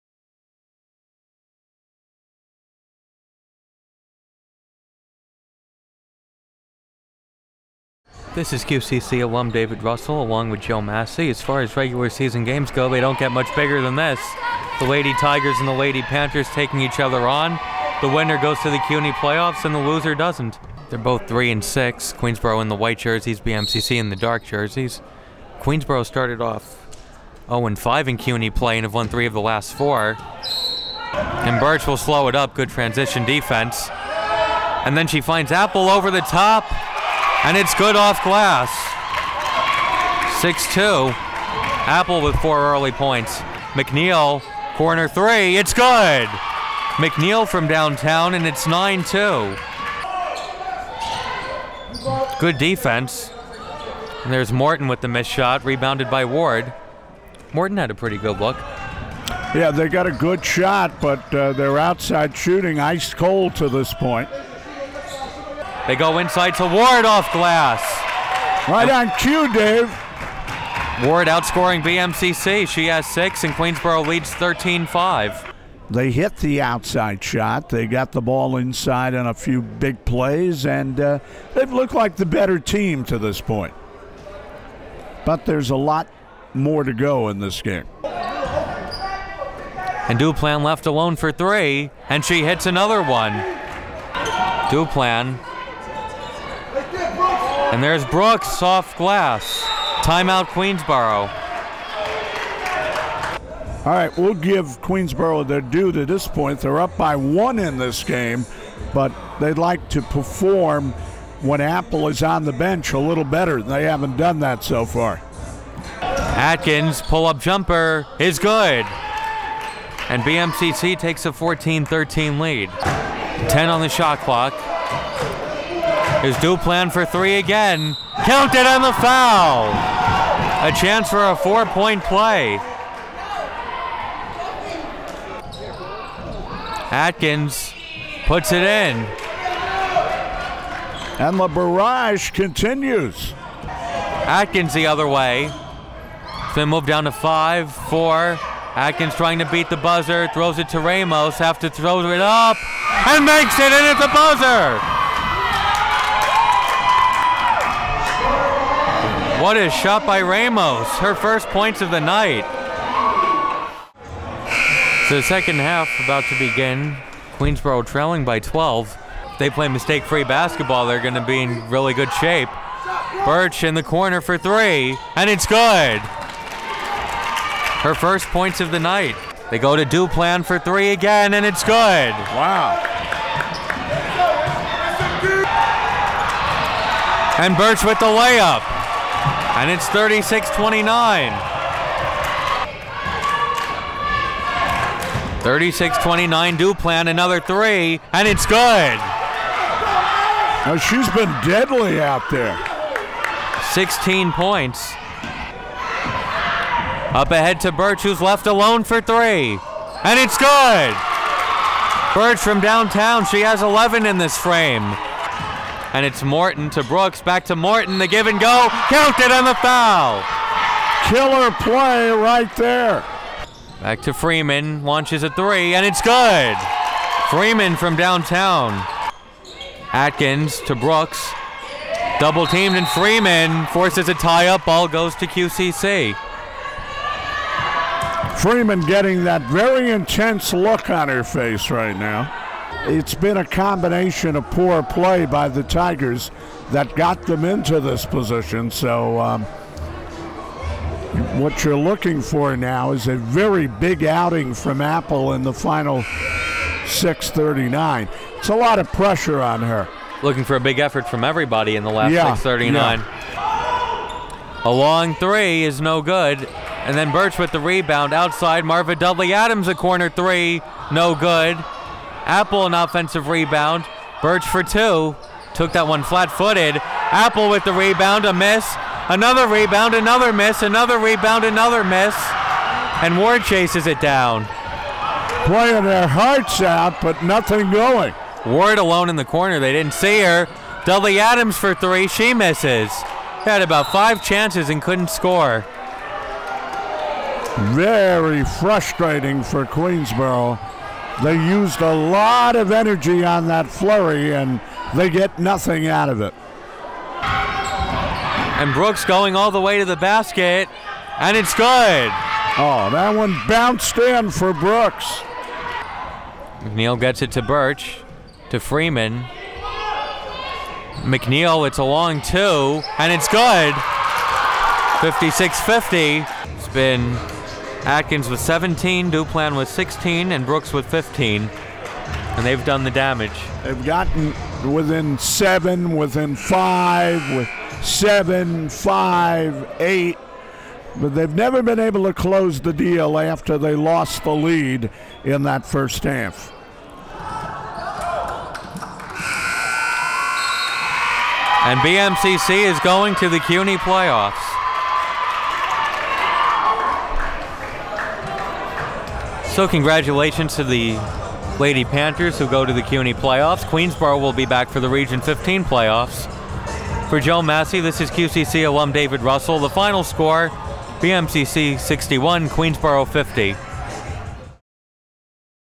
Women's Basketball: Queensborough vs. BMCC (02/09/2016) (Recap)
Final score:
BMCC: 61
Queensborough: 50